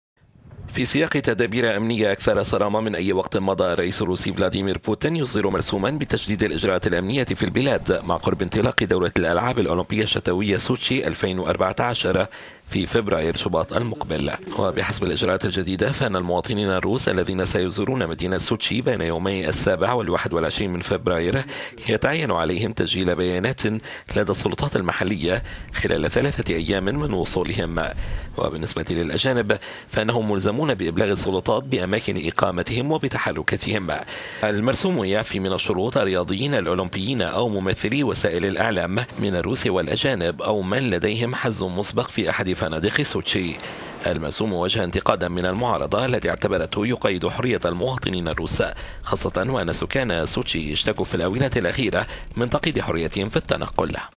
الرئيس الروسي يصدر مرسوما بتشديد الاجراءات الأمنية قبل أوليمبياد سوتشي
في سياق تدابير أمنية أكثر صرامة من أي وقت مضى الرئيس...

في سياق تدابير أمنية أكثر صرامة من أي وقت مضى الرئيس الروسي فلاديمير بوتين يصدر مرسوما بتشديد الاجراءات الأمنية في البلاد
مع قرب انطلاق دورة الألعاب الأوليمبية الشتوية "سوتشي 2014" في فبراير المقبل.

وبحسب الاجراءات الجديدة، فإن المواطنين الروس الذين سيزورون مدينة سوتشي بين يومي 7 و21 فبراير يتعين عليهم تسجيل بيانات لدى السلطات المحلية خلال ثلاثة ايام من وصولهم

 وبالنسبة للأجانب، فإنهم ملزمون ايضا بإبلاغ السلطات بأماكن إقامتهم وبتحركاتهم
المرسوم  يعفى من الشروط الرياضيين الأولمبيين او ممثلي وسائل
 الإعلام من الروس والأجانب أو من لديهم حجز مسبق في احد فنادق سوتشي .